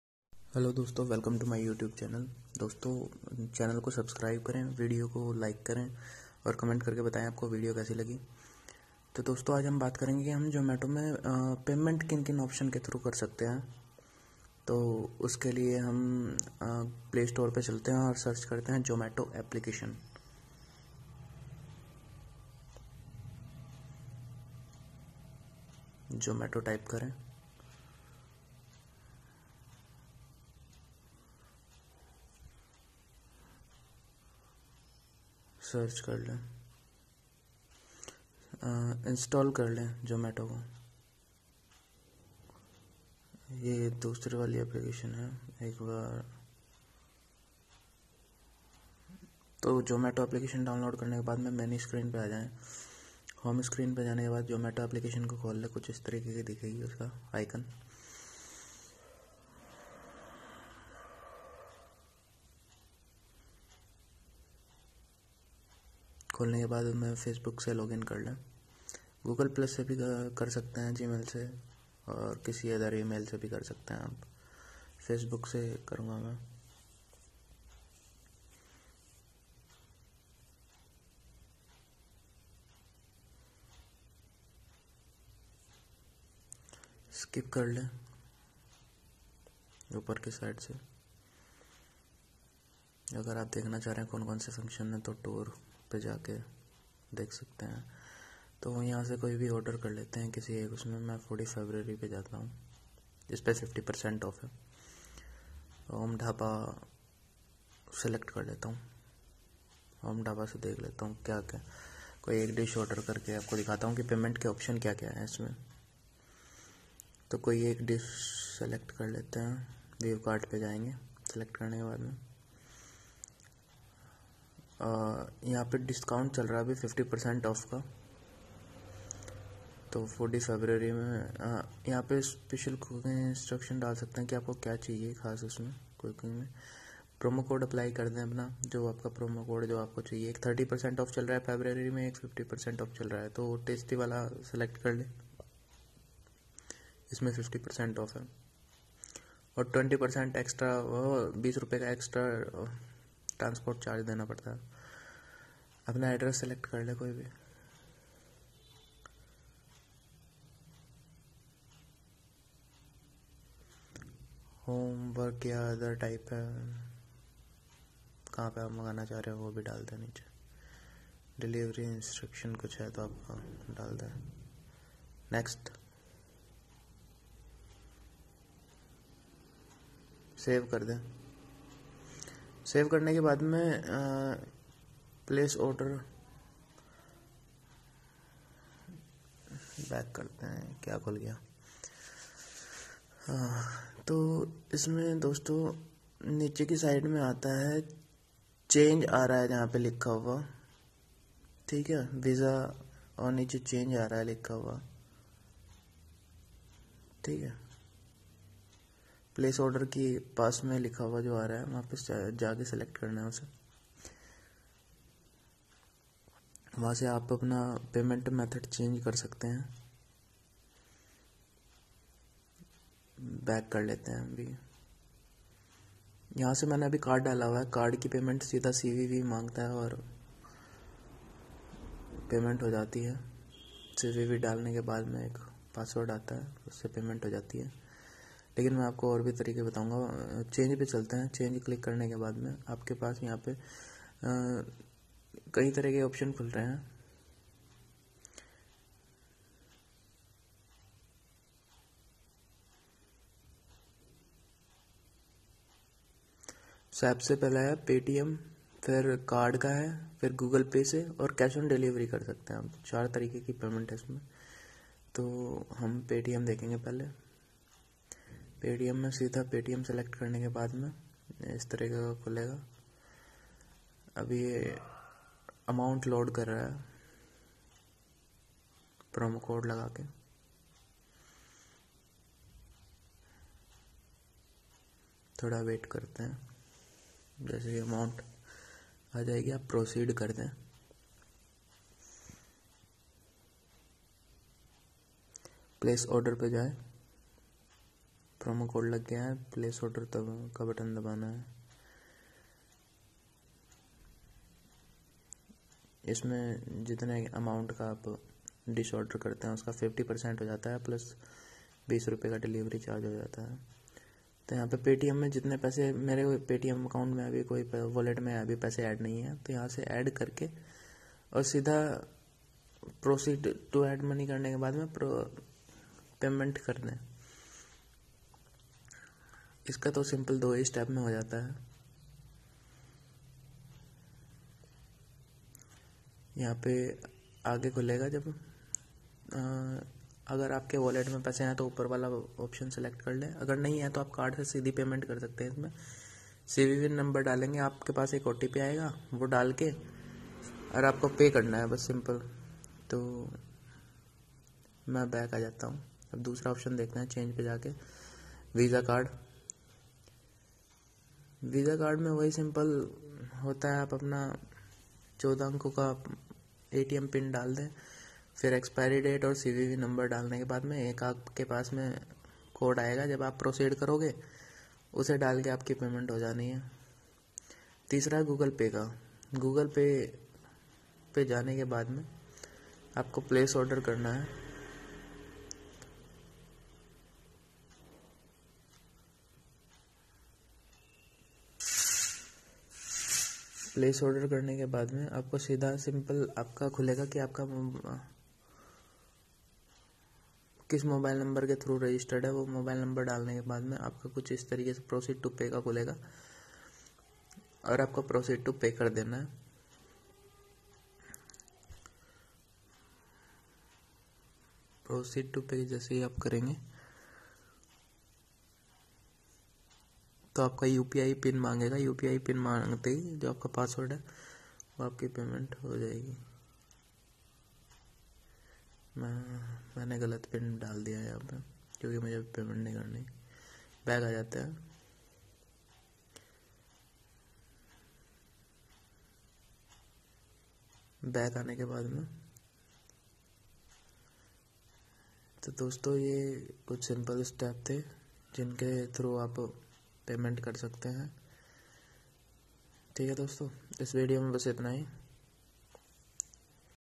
How to order and payment online on Zomato || khana book or payment kaise kre || hindi main
Is video me ham aapko btayenge ki aap zometo se khane ki payment kaise kar sakte h or kya offer chal rha h abhi to janne ke liye ye video jarur dekhe .